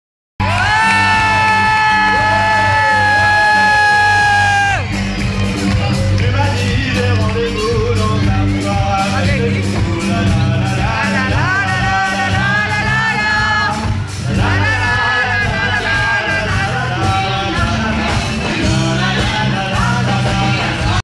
Ambiance de folie à Melun ! Bal des Copains 04/03/07
►GÉNIAL CE BAL A MELUN ! ►1143 participants et 98 courageux bénévoles pour ce 1er "Bal des Copains" ►Merci à vous tous (habitants et partenaires: Copains du Coin / Ville de Melun / Association de commerçants UNICOM / Artistes locaux...